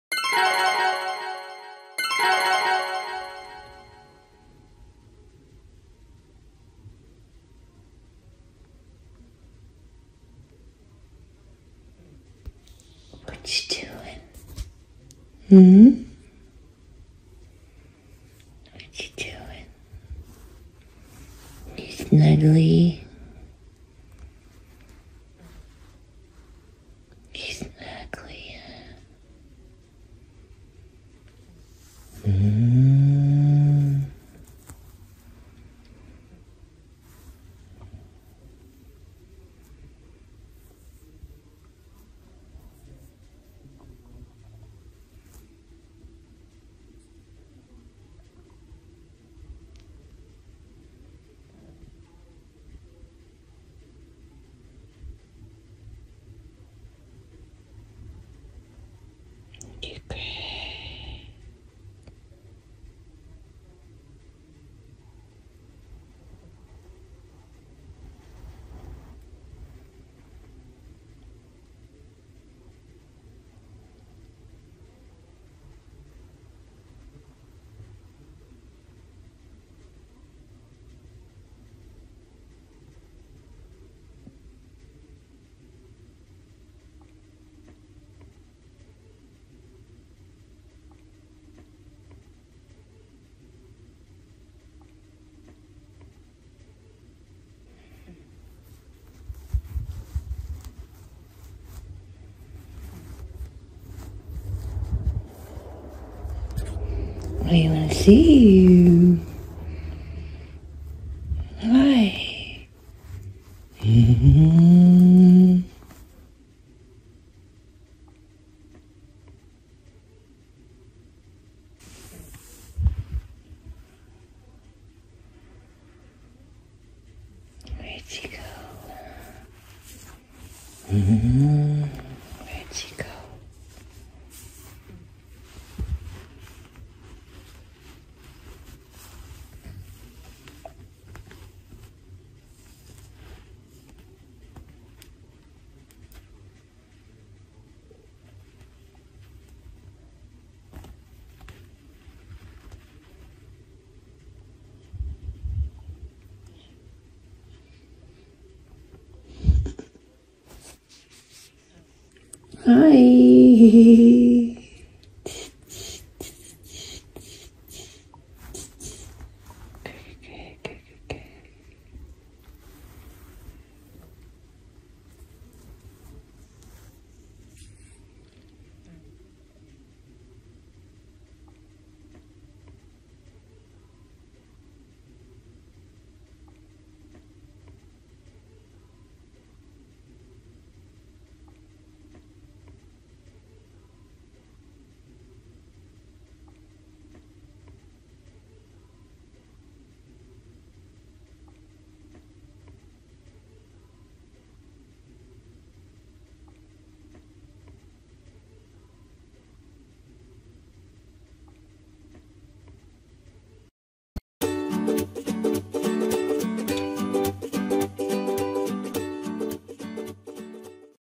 Feeling Shy
Kira, my pet Southern flying squirrel loves to snuggle and tunnel inside Momma's jacket and doesn't want to come out.